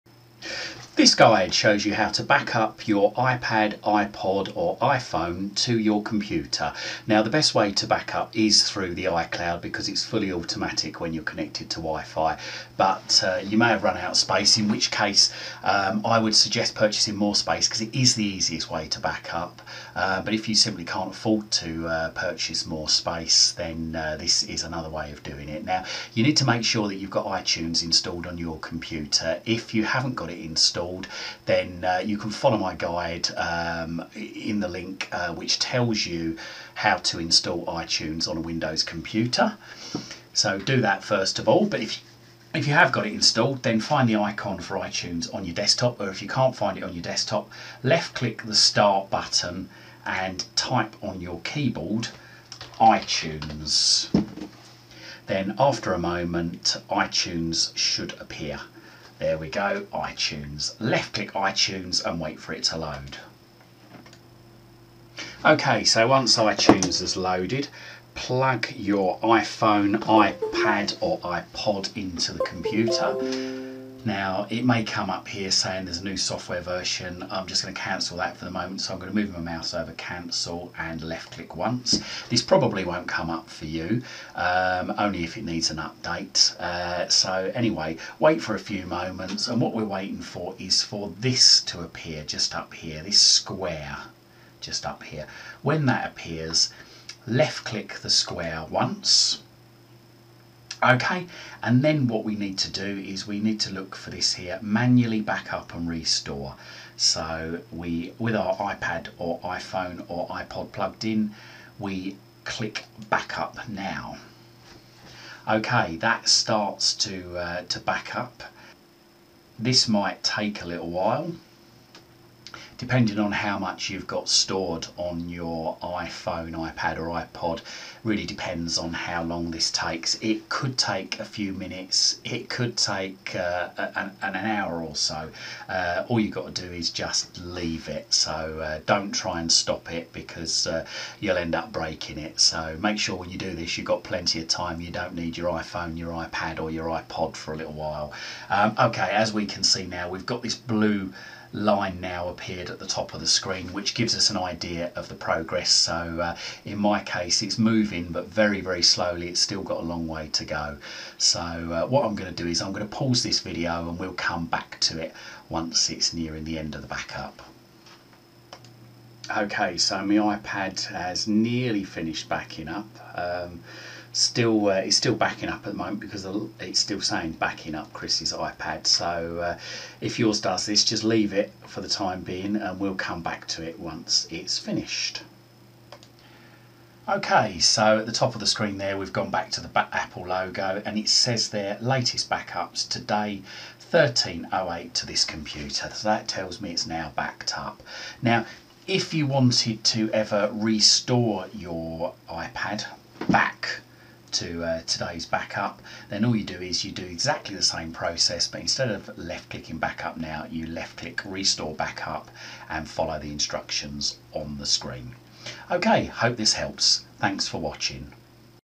Backup Your iPhone iPad or iPod To Your Computer
Shows you how to backup Your iPhone iPad or iPod To Your Computer using iTunes.